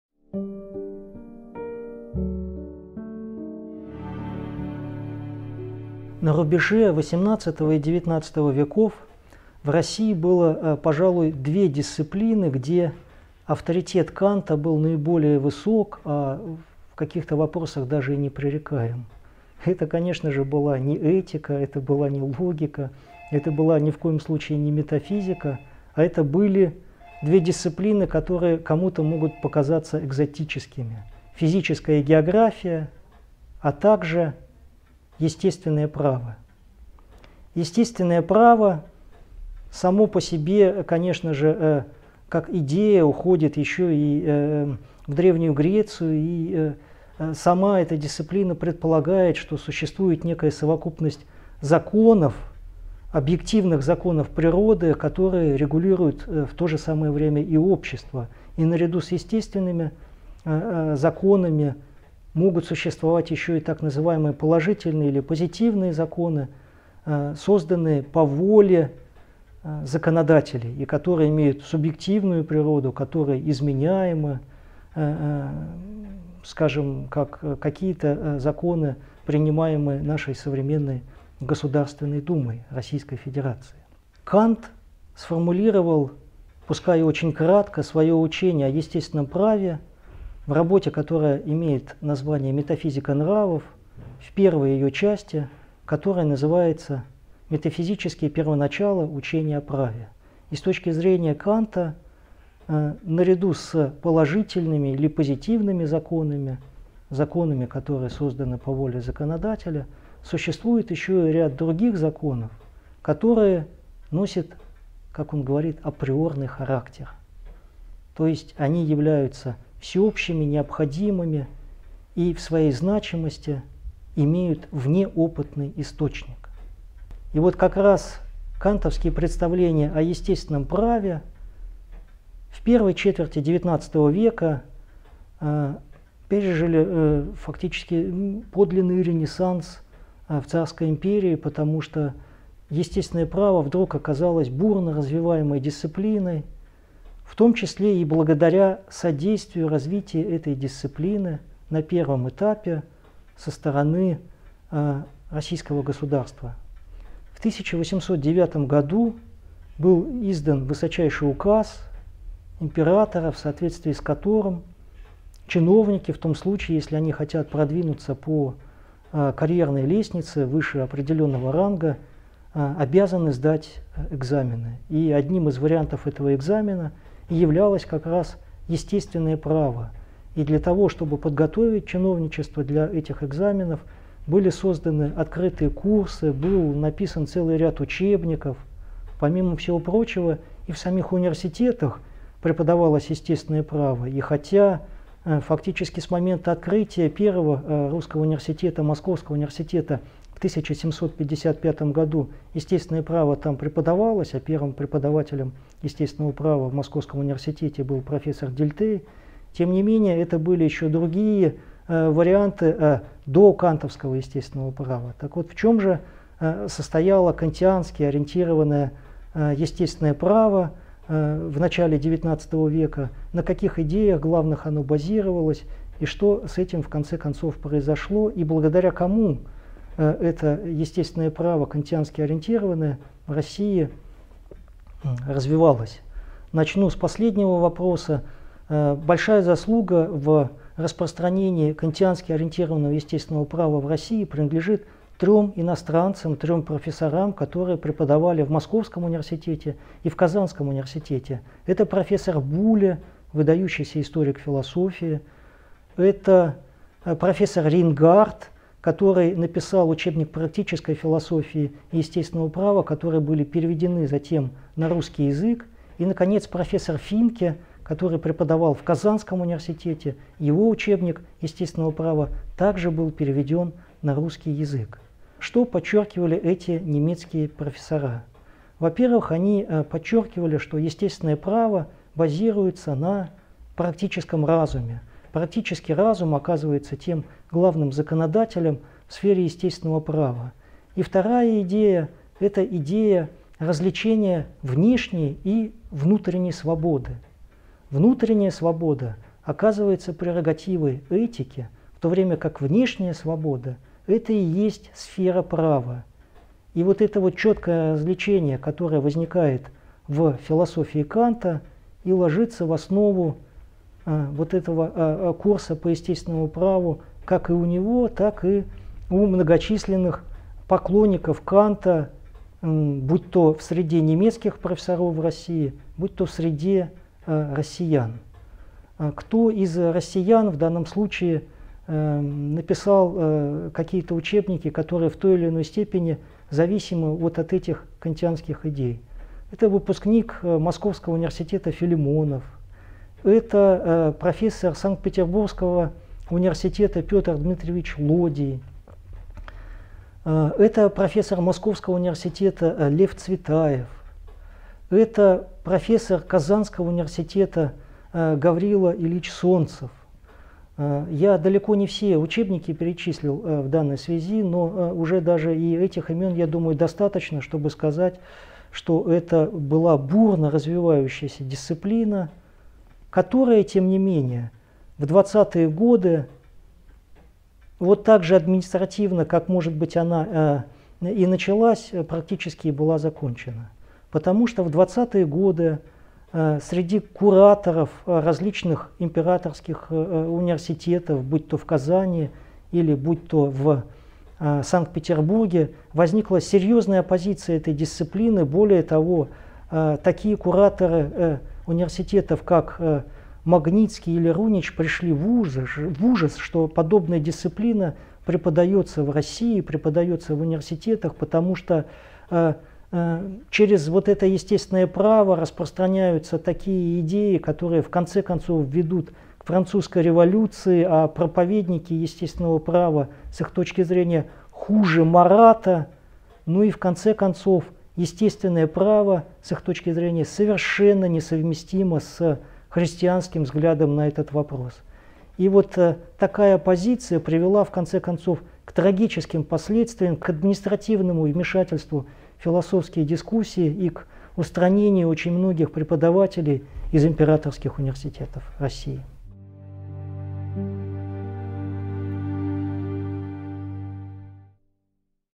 Кант и естесственное право в России. Часть 1
Иммануил Кант в русской культуре

Кант и естественное право в России. Часть 1

Естественное право в России. Рецепция концепции права Канта. Естественное право и православное учение.

Лектор: Круглов Алексей Николаевич - доктор философских наук, профессор РГГУ